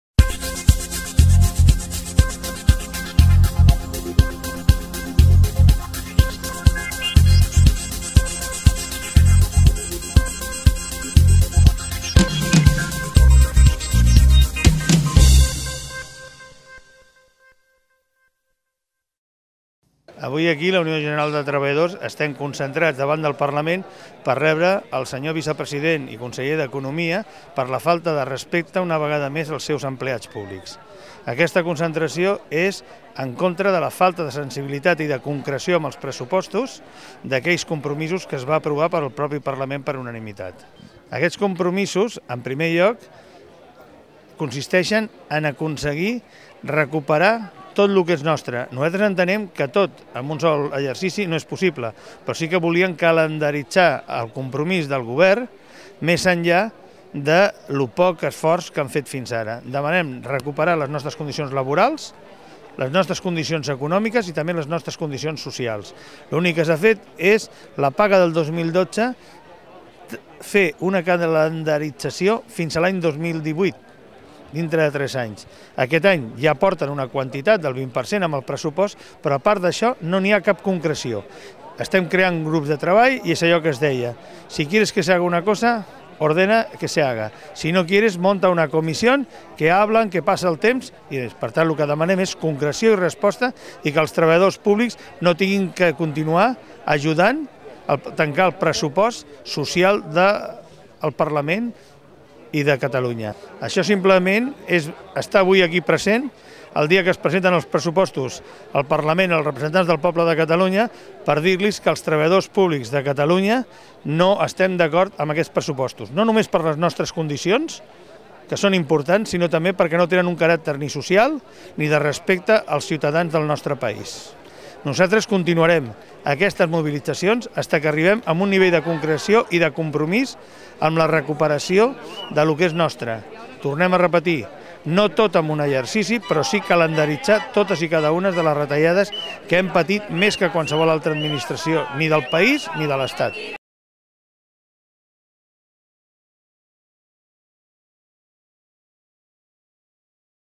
Concentració per uns pressupostos socials
Concentració davant del Parlament de Catalunya dels delegats de la funció pública reclamant uns pressupostos socials i el retorn dels drets laborals perduts.